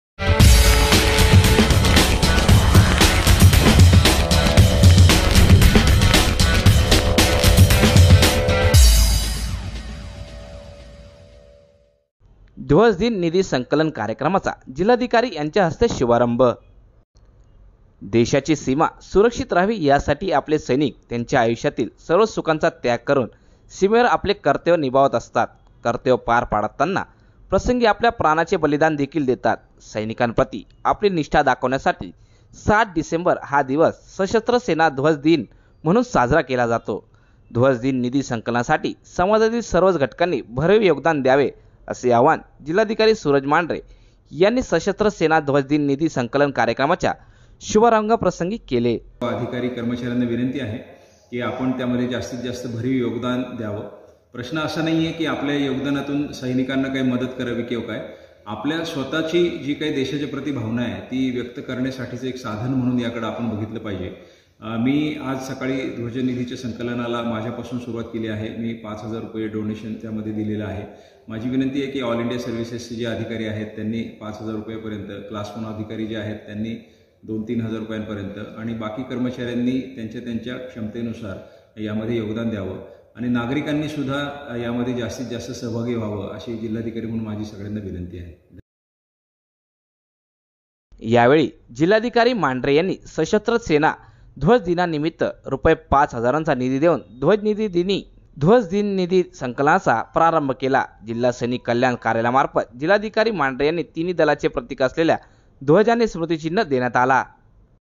Daksh News Live| ध्वजदिन निधी संकलन कार्यक्रमाचा जिल्हाधिकाऱ्याच्या हस्ते शुभारंभ
*ध्वजदिन निधी संकलनास सर्वांनी भरीव योगदान द्यावे*
               *: जिल्हाधिकारी सूरज मांढरे*

*ध्वजदिन 2020 निधी संकलन कार्यक्रमाचा जिल्हाधिकारी यांच्या हस्ते शुभारंभ*

देशाची सीमा सुरक्षित रहावी यासाठी आपले सैनिक त्यांच्या आयुष्यातील सर्व सुखांचा त्याग करून सीमेवर आपले कर्तव्य निभावत असतात. कर्तव्य पार पाडताना प्रसंगी आपल्या प्राणाचे बलिदान देखील देतात. सैनिकांप्रती आपली निष्ठा दाखविण्यासाठी 7 डिसेंबर हा दिवस सशस्त्र सेना ध्वजदिन म्हणून साजरा केला जातो. ध्वजदिन निधी संकलनासाठी समाजातील सर्वच घटकांनी भरीव योगदान द्यावे, असे आवाहन जिल्हाधिकारी सूरज मांढरे यांनी सशस्त्र सेना ध्वजदिन निधी संकलन कार्यक्रमाच्या शुभारंभ प्रसंगी केले आहे.

जिल्हाधिकारी कार्यालयात आज जिल्हाधिकारी सूरज मांढरे यांच्या हस्ते ध्वजदिन 2020 निधी संकलन कार्यक्रमाचा शुभारंभ करण्यात आला. त्यावेळी ते बोलत होते. यावेळी जिल्हा सैनिक कल्याण अधिकारी माजी लेफ्टनंट कमांडर ओंकार कापले, जिल्हा सैनिक कार्यालयातील कर्मचारी नवनाथ दाने, संजय थोरात, सुनिल पाटील आदी उपस्थित होते.

जिल्हाधिकारी श्री. मांढरे म्हणाले, 1949 पासून प्रत्येक वर्षी 07 डिसेंबर रोजी सशस्त्र सेना ध्वजदिन साजरा करण्यात येतो. जिल्हा सैनिक कल्याण कार्यालयामार्फत या दिवसापासून ध्वजदिन निधी संकलनास प्रारंभ करण्यात येत असतो. संकलित करण्यात येणाऱ्या निधीचा वापर धारातीर्थी पडलेल्या तसेच अपंगत्व आलेल्या सैनिक, युद्धविधवा, माजी सैनिक व त्यांच्या कुटूंबाच्या कल्याणासाठी करण्यात असतो. या निधीच्या माध्यमातून आपली देशाच्याप्रती असलेली भावना व आदर व्यक्त करण्यासाठीचे साधन म्हणून ध्वजदिन निधी संकलनात सर्वांनी योगदान देणे आवश्यक आहे. त्यासाठी भारतीय प्रशसकीय सेवेतील अधिकारी यांनी रूपये पाच हजार, वर्ग 1 अधिकारी यांनी दोन ते तीन हजार व इतर अधिकारी कर्मचारी यांनी त्यांच्या क्षमतेनुसार योगदान द्यावे, असे जिल्हाधिकारी श्री. मांढरे यांनी यावेळी सांगितले.

गेल्यावर्षी जिल्ह्याला शासनाकडून ध्वजदिन निधी संकलासाठी 1 कोटी 29 लाख 48 हजार रुपयांचे उद्दीष्ट देण्यात आले होते. जगावर कोरोना विषाणूचा प्रादुर्भाव असतांना देखील जिल्ह्यातील सर्वच घटकांनी या निधीसाठी संकलन करून दिलेल्या उद्दीष्टापैकी 54.81 टक्के म्हणजेच रूपये 70 लाख 97 हजार 200 इतका निधी संकलित केला आहे. यावर्षी देखील शासनाकडून आपल्या जिल्ह्याला 1 कोटी 29 लाख 48 हजार रुपयांचे उद्दीष्ट देण्यात आले आहे. हे उद्दीष्ट आपण सर्वच साध्य करू असा विश्वास  यावेळी जिल्हाधिकारी श्री. मांढरे यांनी व्यक्त केला.

यावेळी जिल्हाधिकारी श्री. मांढरे यांनी सशस्त्र सेना ध्वजदिना निमित्त रूपये पाच हजारांचा निधी देवून  धजदिन निधी संकलानाचा प्रारंभ केला. जिल्हा सैनिक कल्याण कार्यालयामार्फत जिल्हाधिकारी श्री. मांढरे यांना तिन्ही दलाचे प्रतिक असलेला ध्वज आणि स्मृतीचिन्ह देण्यात आले